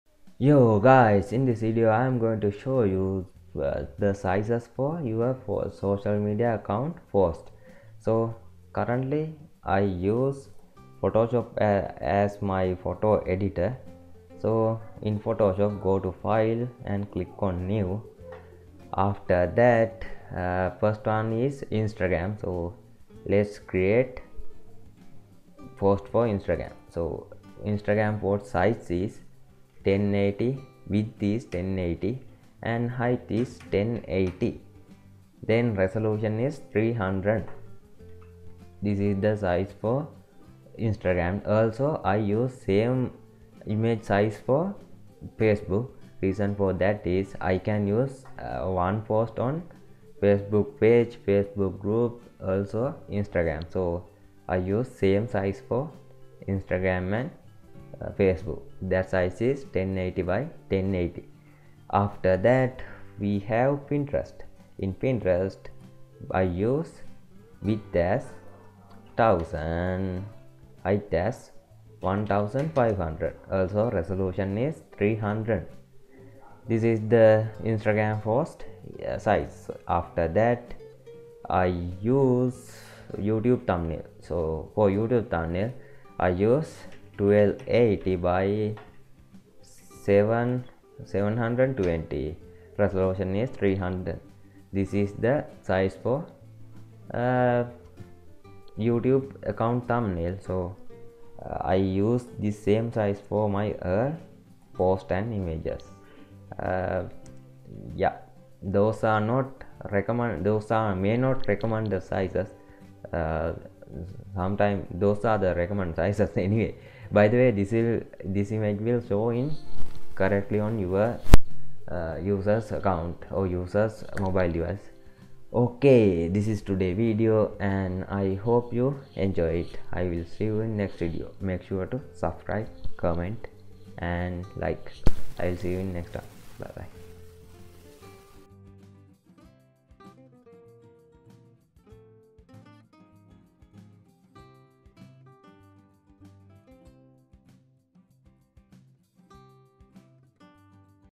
Social Media Design Sizes - Instagram Facebook & Youtube Thumbnail Size
Yooo guys...

😏 Today I am going to show Social Media Design Sizes - Instagram Facebook & Youtube Thumbnail Size
😏 It is a small video.
😏 You will find post sizes for,
 ➜ Instagram post size
 ➜ Facebook post size
 ➜ Pinterest post size
 ➜ Youtube thumbnail size

😏 I use Photoshop for design thumbnail.

Buy Hosting & Domain On Namecheap & Save up to 86% with Namecheap  Domain & Shared Hosting package deal!